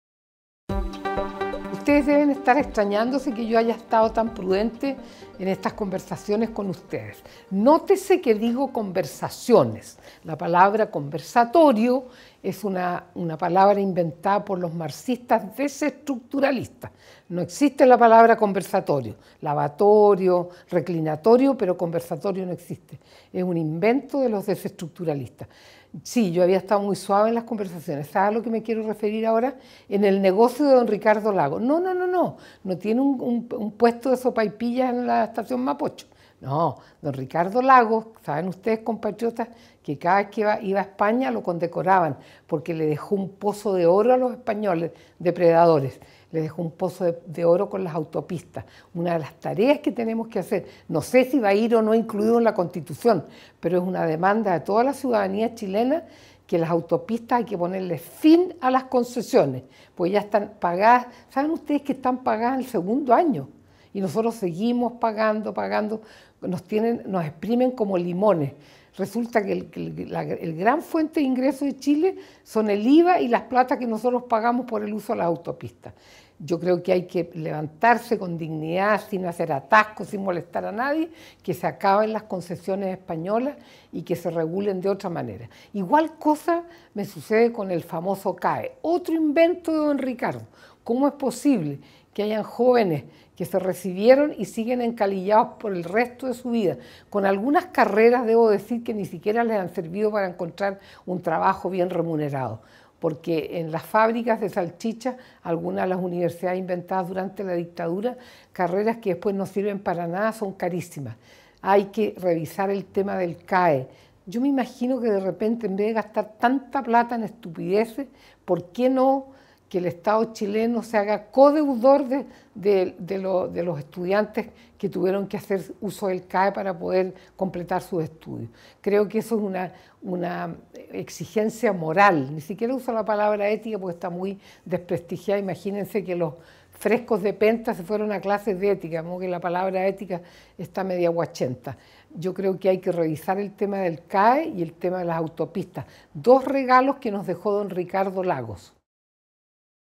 Fin al negociado de las Autopistas y sus concesiones
Es insólito, que sigamos pagando las autopistas, cuando ya se pagaron hace rato.
NO + TAG ¡¡¡¡¡¡
Chile para los  Chilenos